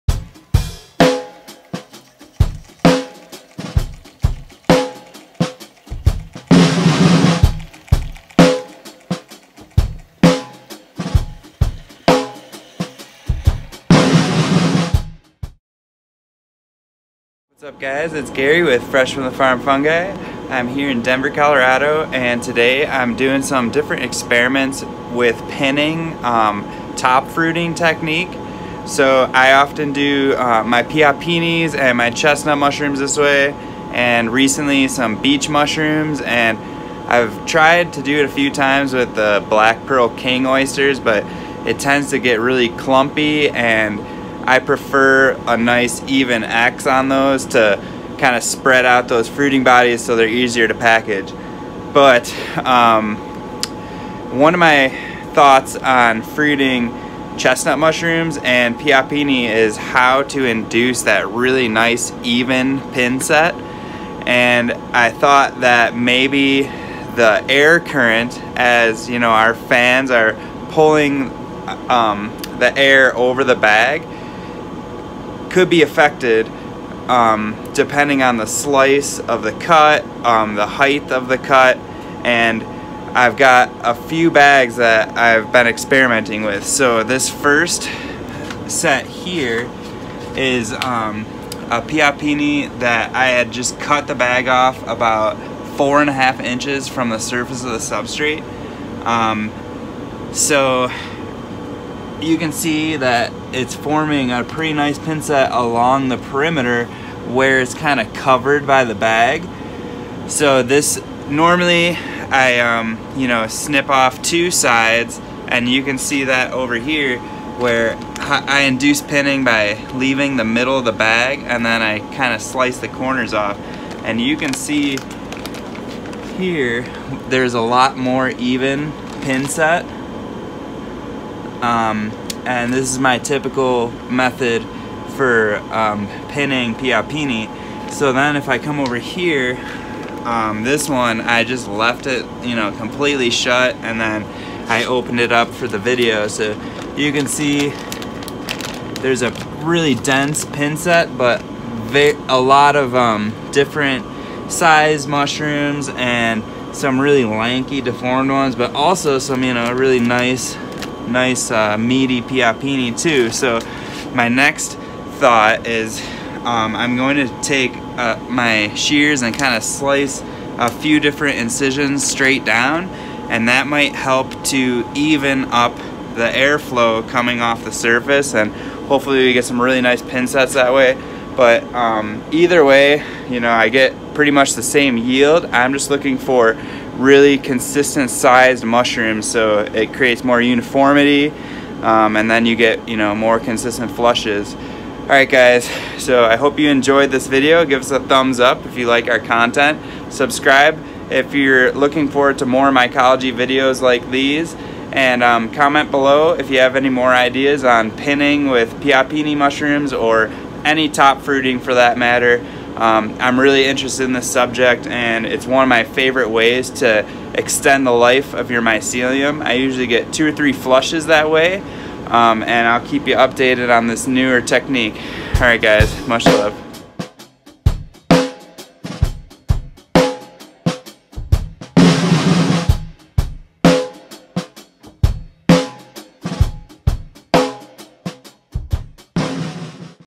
Pinning experiment: fresh air exchange and best way to start mushrooms
This video is about Pinning experiment: fresh air exchange - we trial 3 different techniques for top fruiting pioppino mushrooms.  to purchase our living mycelium cultures visit our ETSY shop FreshFungi MUSHLOVE

Gary's Recommended Mushroom Gear and Grow Equipment